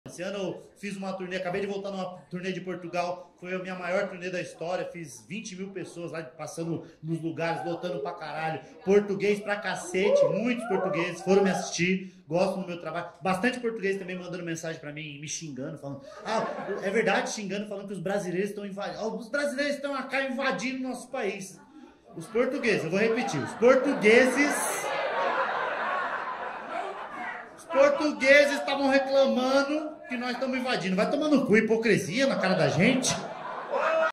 Afonso Padilha Os portugueses tavam reclamando
Ao vivo no primeiro bar de comédia do Brasil.

Reserve seu lugar e compre seus ingressos em nosso site!